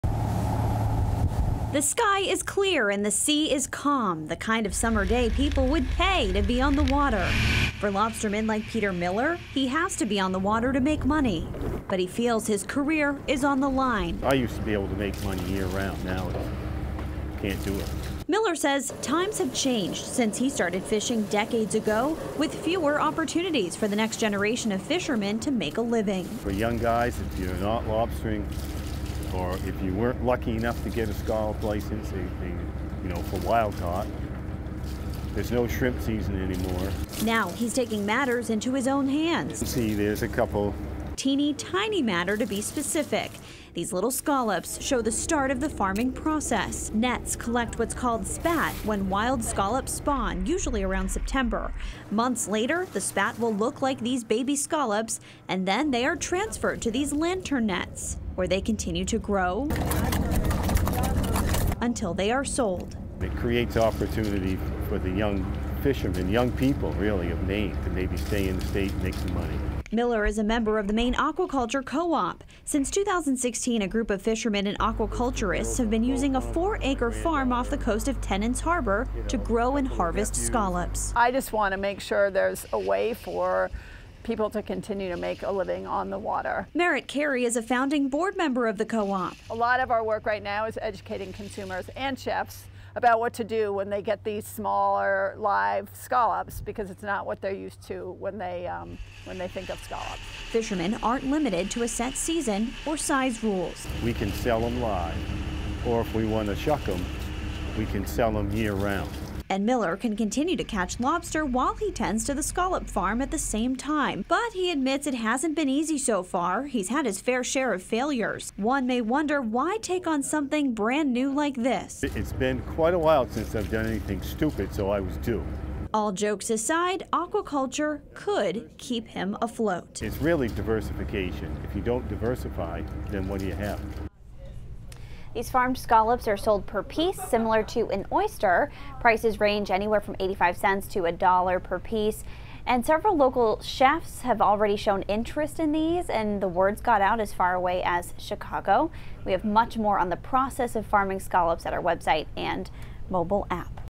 Money in scallop farming in Maine
Scallop farming is helping to feed a high demand in Maine and bring in money for fishermen in the state.